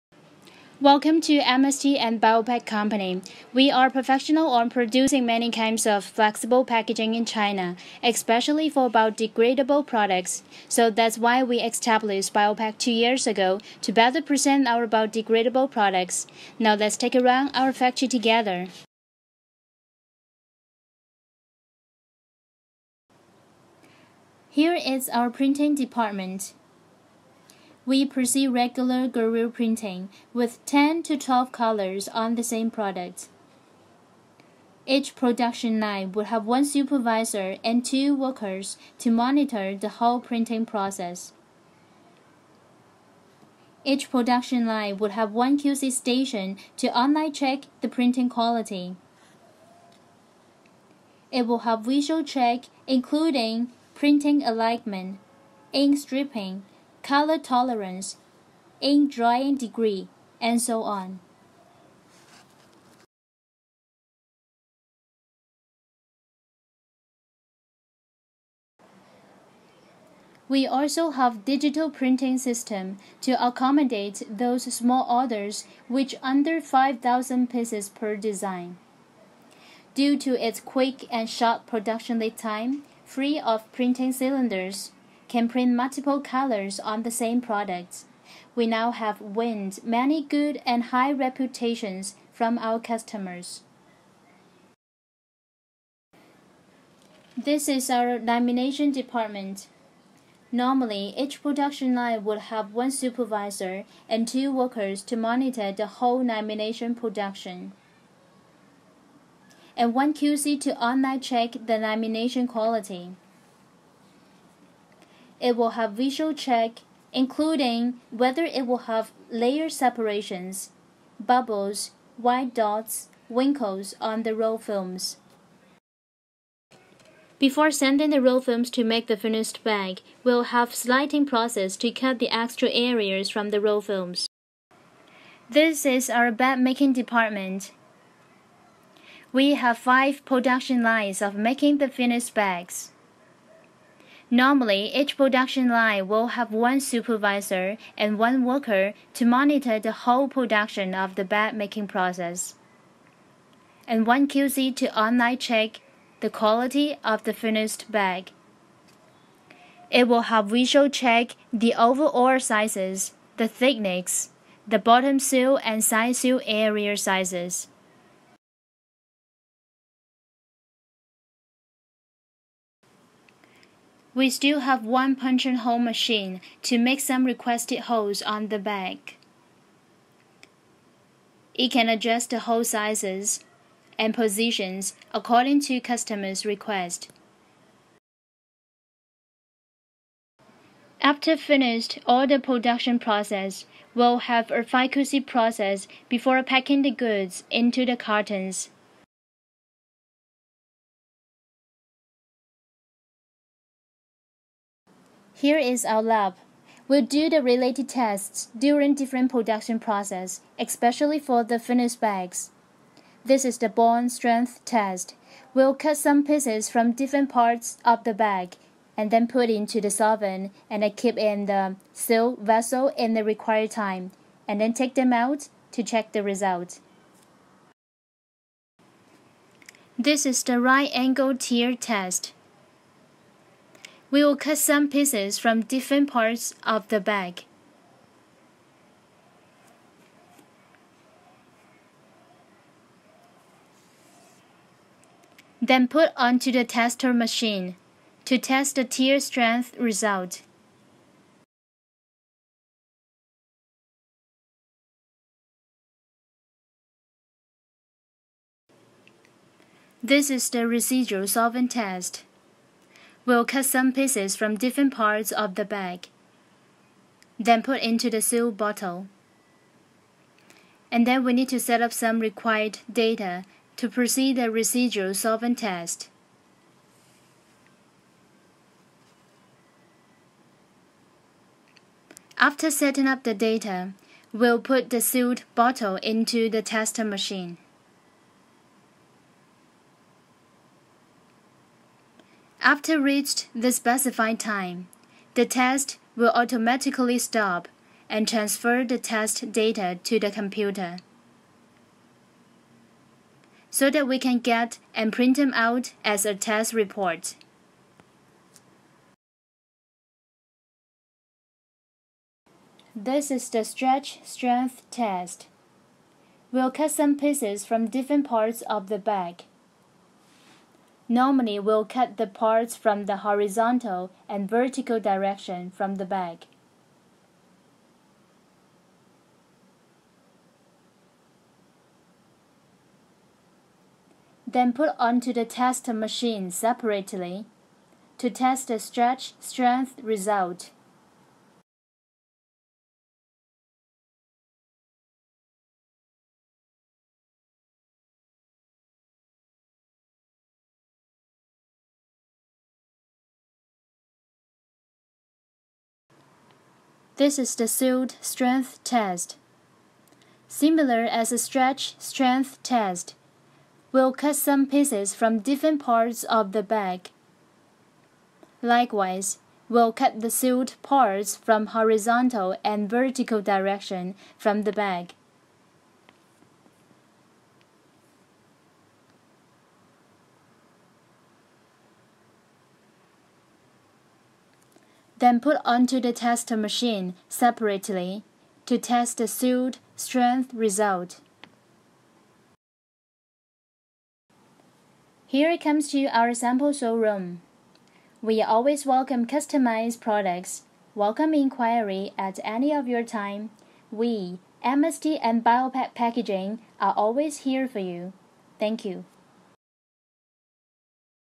Printing and testing of compostable bags| Bio Back Factory Tour | Biodegradable bags stretching
One such business is a biodegradable plastic bag manufacturing business. The biodegradable plastic bag manufacturing business. In this video, we will give a tour of our printing HP  Indigo 20000 digital press, and also we will take you around our stretching test labs.
Compostable Bags Manufacturing.
Compostable Bags printing machines
Compostable Bags stretching test lab
Compostable bags factory.